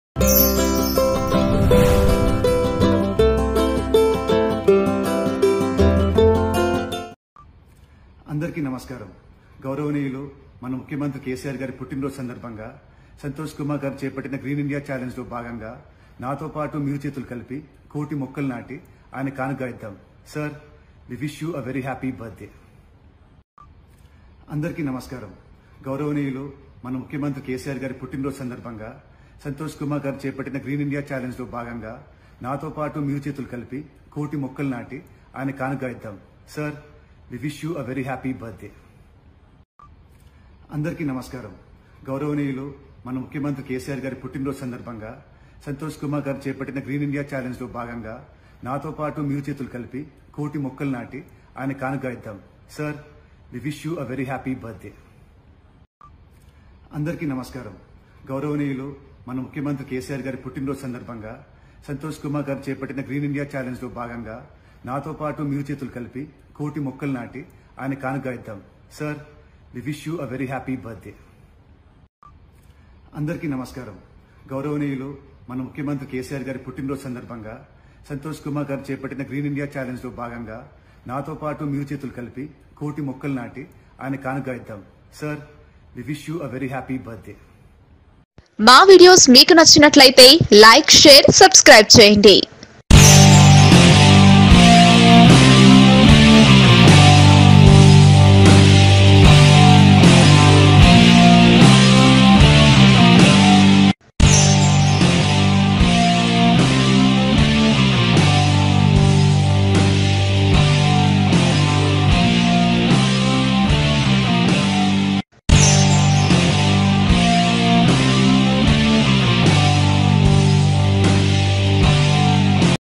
Rang De Hero Nithin Special Birthday Gift To CM KCR | Birthday Wishes To TRS CM KCR | Tollywood News
Rang De Hero Nithin Special Birthday Gift To TRS CM KCR, exclusively on News Mantra.